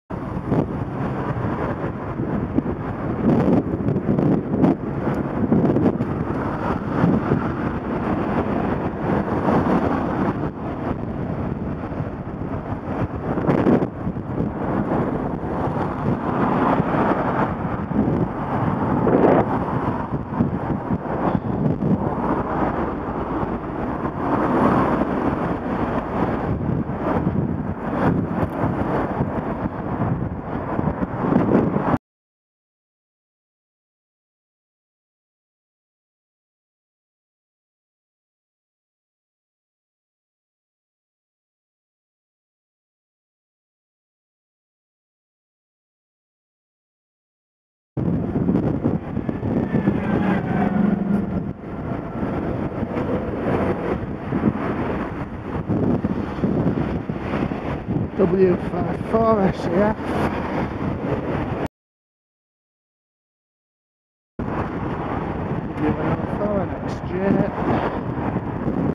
W54SEF and WN04NXJ - two double close passes
Bear with me on this one. Both of these drivers passed me far too close, twice each. On the first stretch of road, I'm used to people doing stupid things, with there being half a cycle lane the full length. The second piece of road I'm keeping out of the door zone, both passes were far too close, the Clio being the worse of the two